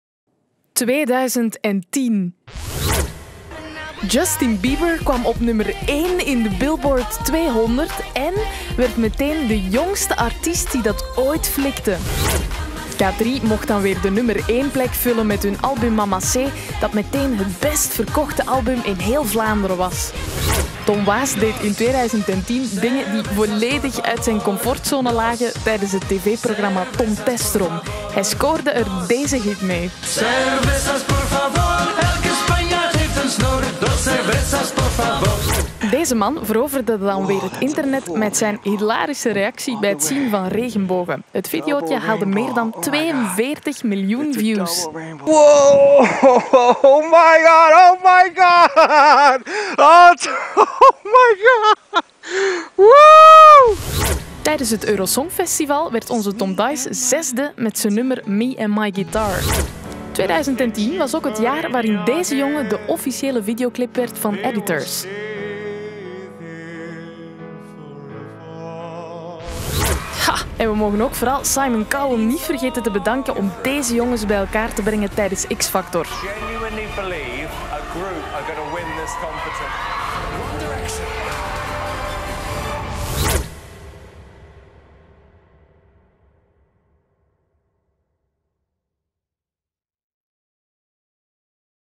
MNM: Dit was 2010!
Wist je nog dat One Direction in 2010 door Simon Cowell bij elkaar werd gebracht tijdens X-factor én herinner je je de man die ongelooflijk enthousiast reageerde op een regenboog?